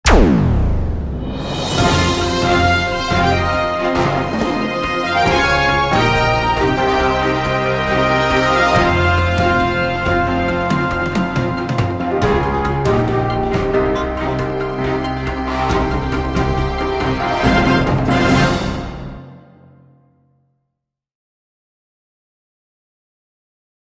The new intro to my channel!
THIS IS IT, FOLKS! DanteTube is getting a revamp! And with it, a new intro! Same logo, same channel name, and same music.

- DBR

Animation software: Keynote
Editing software: iMovie
Music making softwares: Soundtrap and Audacity